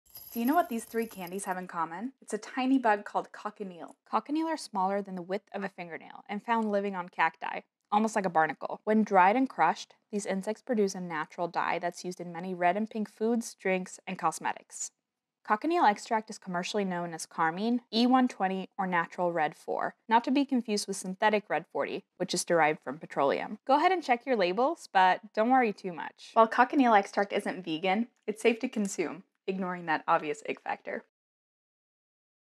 Food Secrets | Red Food Dye
Among one of the biggest concerns is that some dyes, namely Red 40 and Yellow 5 and 6, are believed to be carcinogens. This is because they are made with benzene, a well-known carcinogen. Most other countries have banned benzene entirely due to the effects it has on the health of people who choose to consume it.